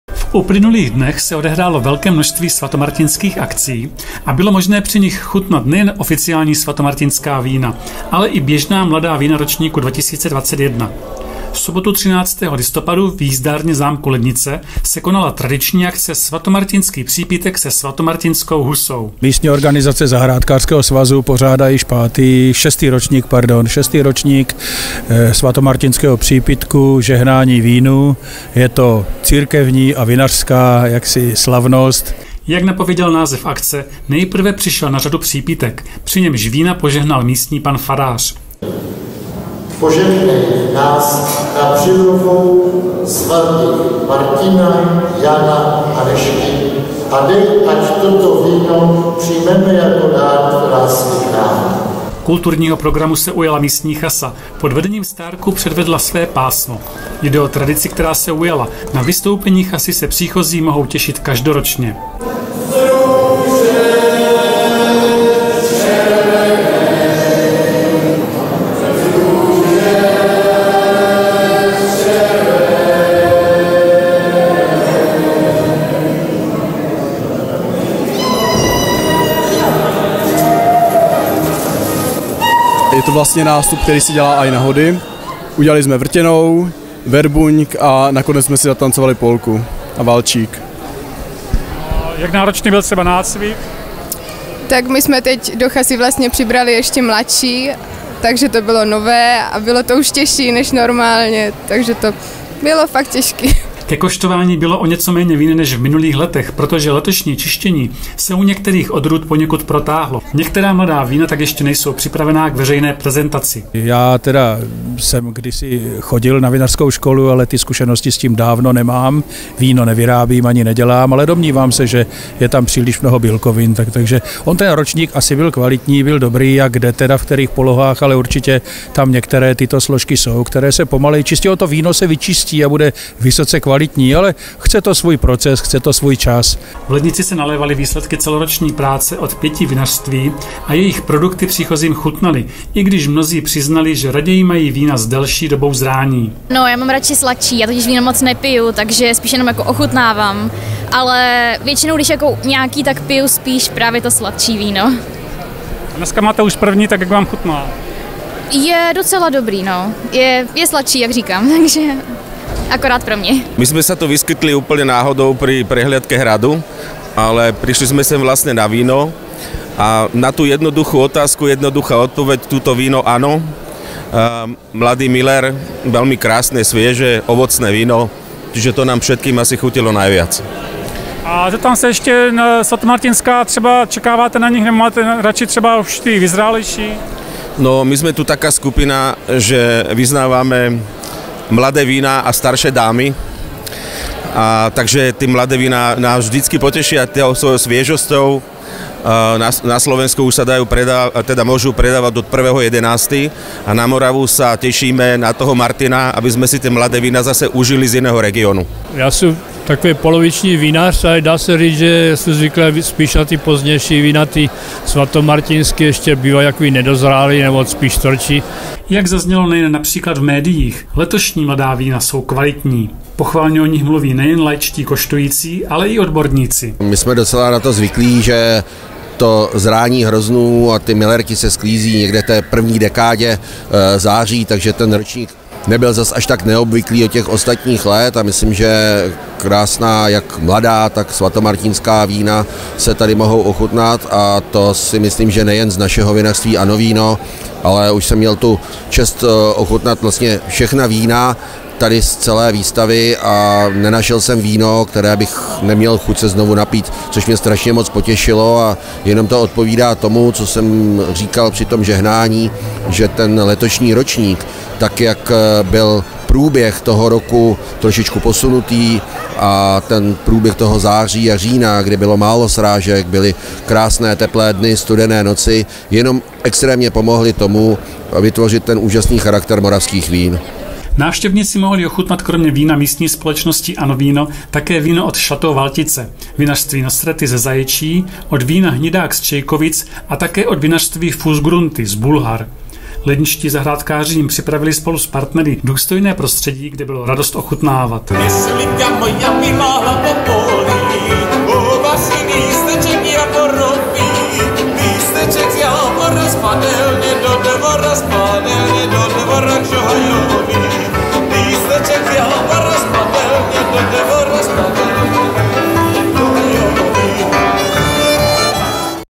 V Lednici si připili Svatomartinským vínem k pečené husičce
V uplynulých dnech se odehrálo velké množství svatomartinských akcí a bylo možné při nich chutnat nejen oficiální Svatomartinská vína, ale i běžná mladá vína ročníku 2021. V sobotu 13. listopadu v jízdárně zámku Lednice se konala tradiční akce- Svatomartinský přípitek se svatomartinskou husou.